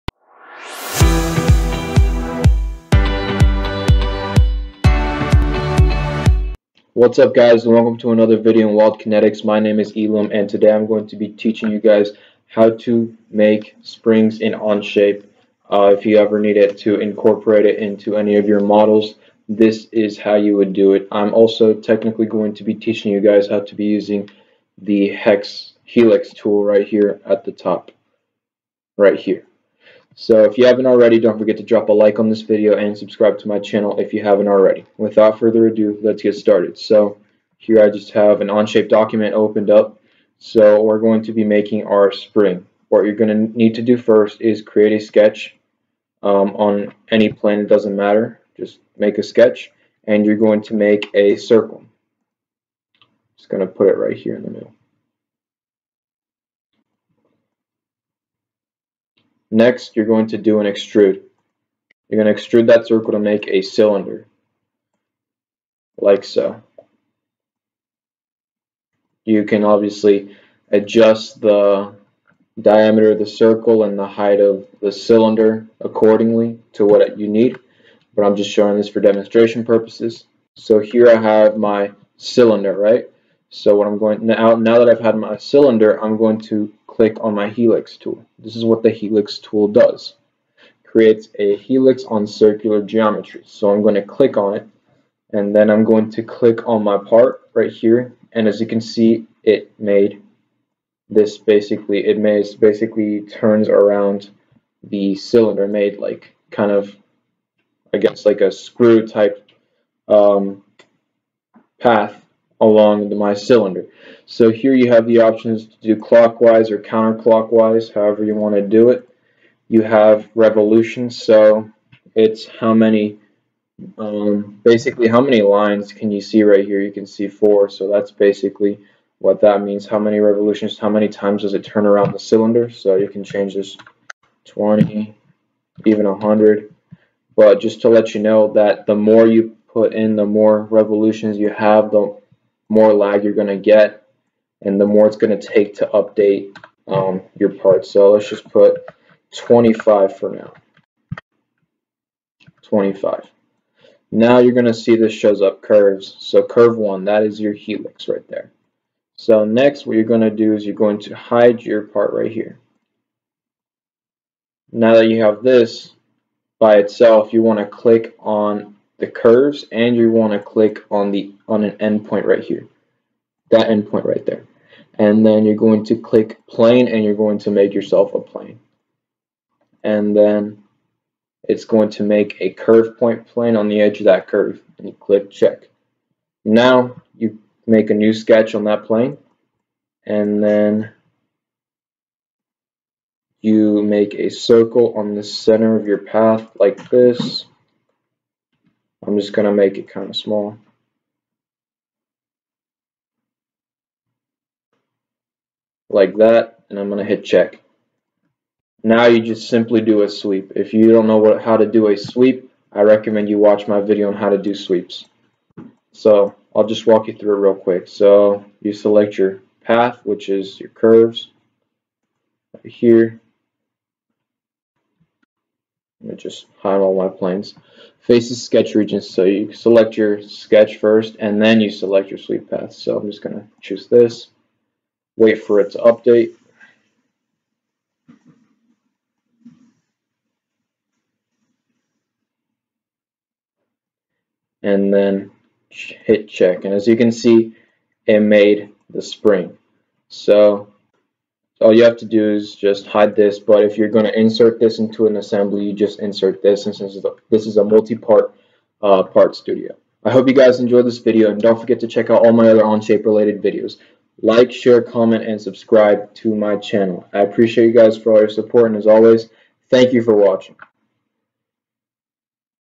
How to Model A Spring in Onshape
In this video, I will teach you how to model a spring in Onshape using a construction cylinder.